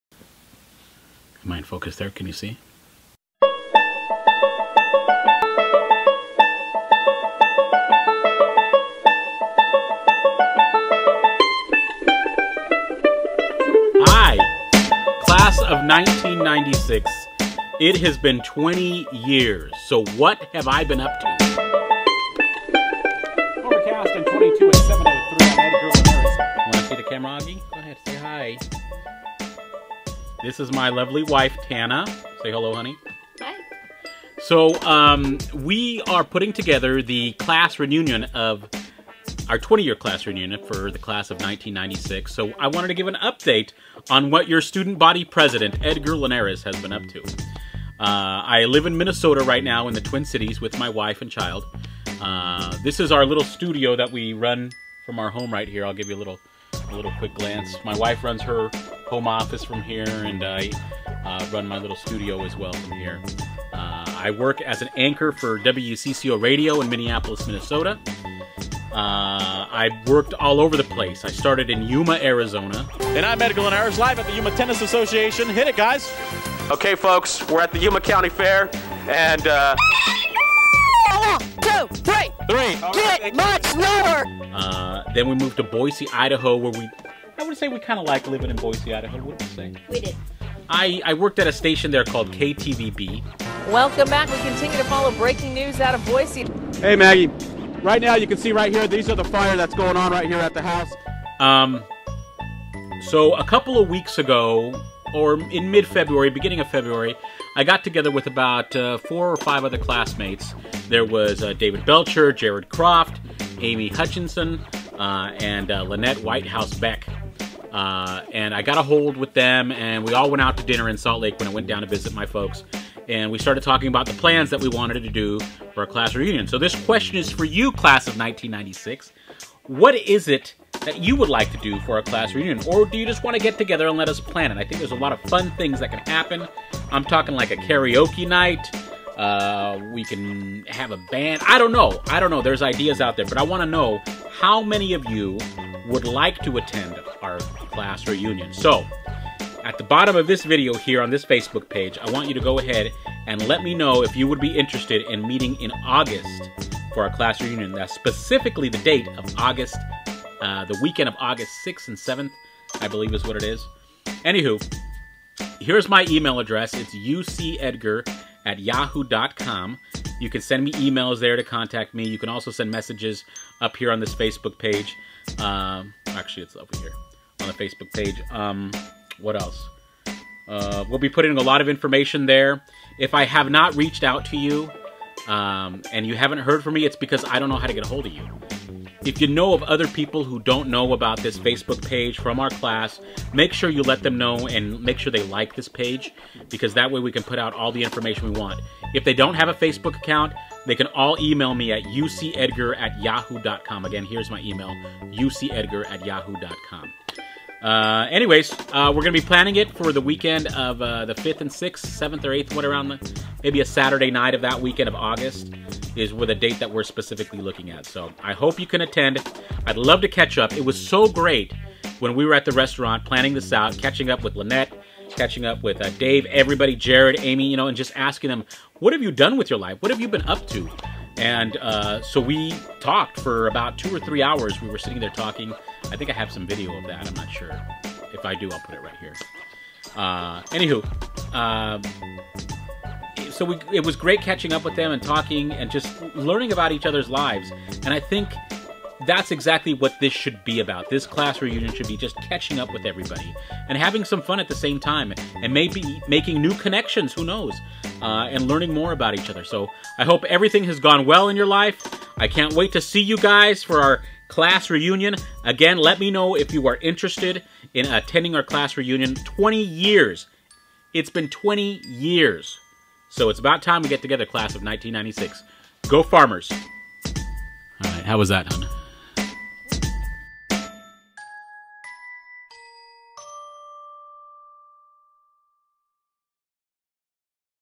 MNReporter Vlog#4: Granite High School Class of 1996
This Vlog #4 is for Salt Lake City, Utah's Granite High School class of 1996. We are gathering for our 20th Class reunion this year. We're looking at Saturday, August 6th for the date.

For more information follow our Facebook page and let us know if you're interested in attending.